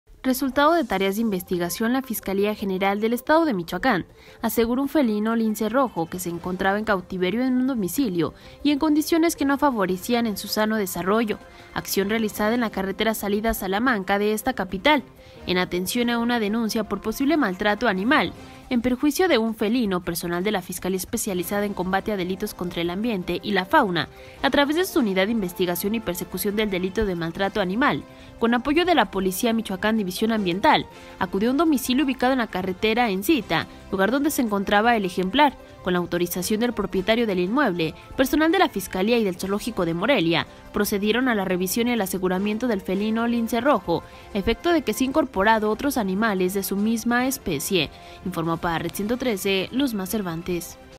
Rescata FGE a un ejemplar de lince rojo en cautiverio
+La acción fue realizada en un inmueble ubicado en la carretera salida Salamanca

Morelia, Mich.- 18 de septiembre de 2020.- Resultado de tareas de investigación, la Fiscalía General del Estado de Michoacán (FGE), aseguró un felino, lince rojo, que se encontraba en cautiverio en un domicilio y en condiciones que no favorecían en su sano desarrollo; acción realizada en la carretera salida Salamanca de esta capital.

En atención a una denuncia por posible maltrato animal, en perjuicio de un felino, personal de la Fiscalía Especializada en Combate a Delitos Contra el Ambiente y la Fauna a través de su Unidad de Investigación y Persecución del Delito del Maltrato Animal, con apoyo de la Policía Michoacán División Ambiental, acudió a un domicilio ubicado en la carretera en cita, lugar donde se encontró al ejemplar.

Con la autorización del propietario del inmueble, personal de la Fiscalía y del Zoológico de Morelia procedieron a la revisión y al aseguramiento del felino lince rojo, a efecto de que sea incorporado con otros animales de su misma especie.